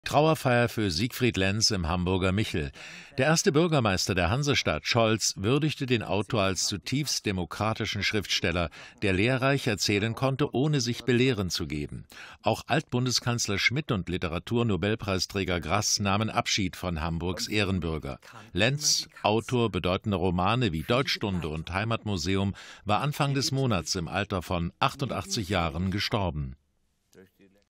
Trauerfeier für den in Ostpreußen geborenen Hamburger Ehrenbürger Siegfried Lenz
Hamburg nimmt Abschied von seinem Ehrenbürger Siegfried Lenz. Geboren wurde er 1926 im ostpreußischen Lyck.

Altmodisch, aber erfolgreich/Zum Tod von Siegfried Lenz, der Ostpreußen nach dem Krieg eine Stimme gab:

Kritisch, konstruktiv - Klartext für Deutschland